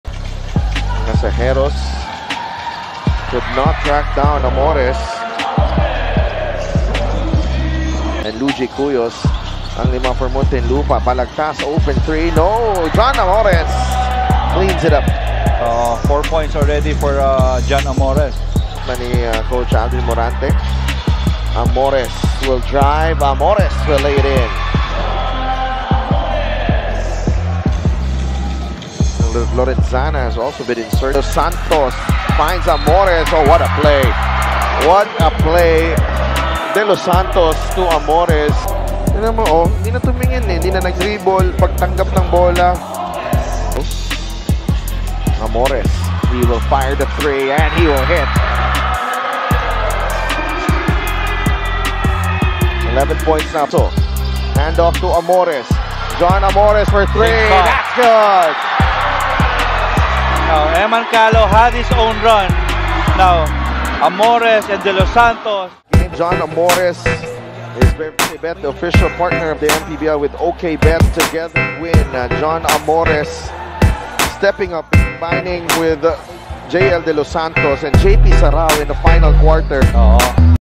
John Amores | Best Player of The Game | 16 PTS 7 REB 2 AST | Aug. 17, 2023
Muntinlupa Cagers vs. Makati Okbet Kings